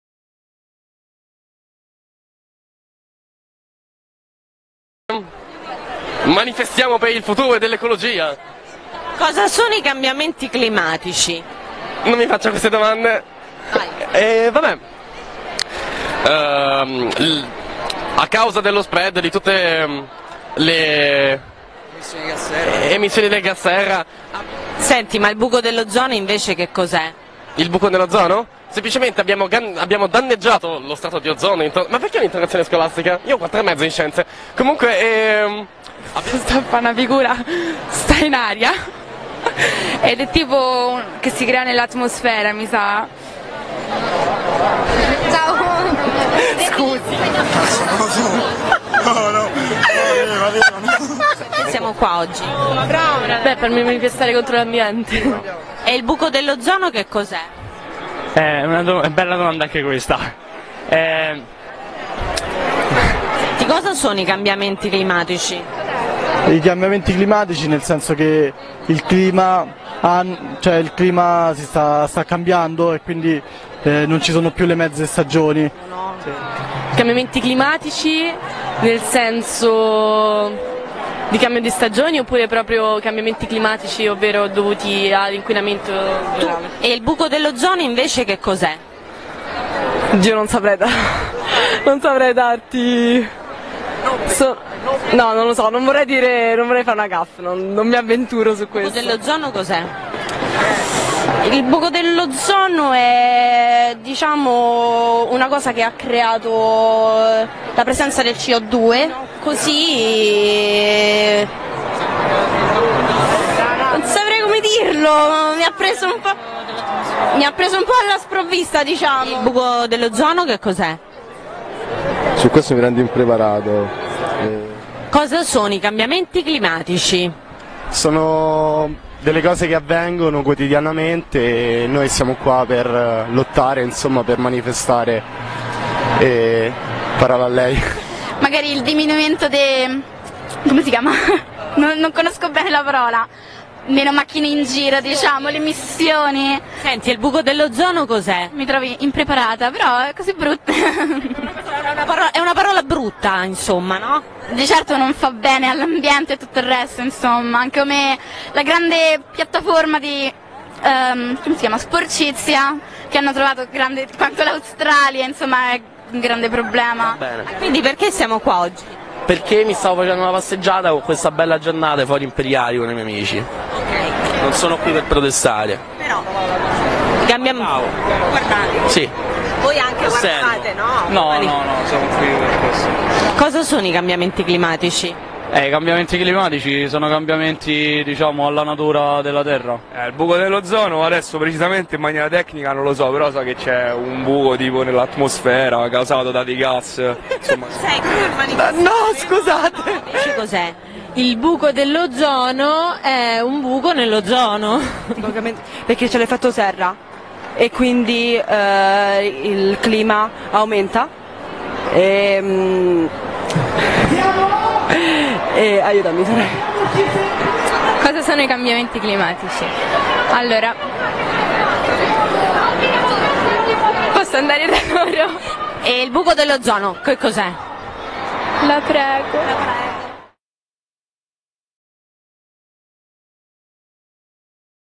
Manifestazione per l'ambiente
Manifestazione dei giovani in favore dell'ambiente. Luogo: Roma